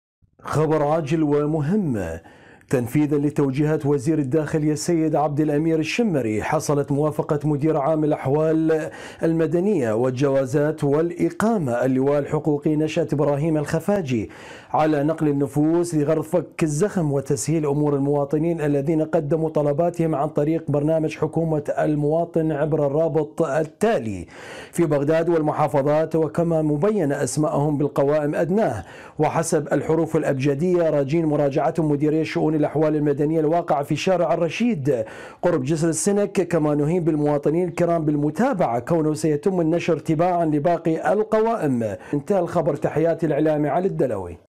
اسماء وجبه جديده نقل النفوس في بغداد والمحافظات حسب ما اعلنته مديرية الاحوال تحياتي علي الدلوي
عاجل اسماء وجبه جديده نقل النفوس في بغداد والمحافظات حسب ما اعلنته مديرية الاحوال المدنيه والجوازات والاقامه تحياتي الاعلامي علي الدلوي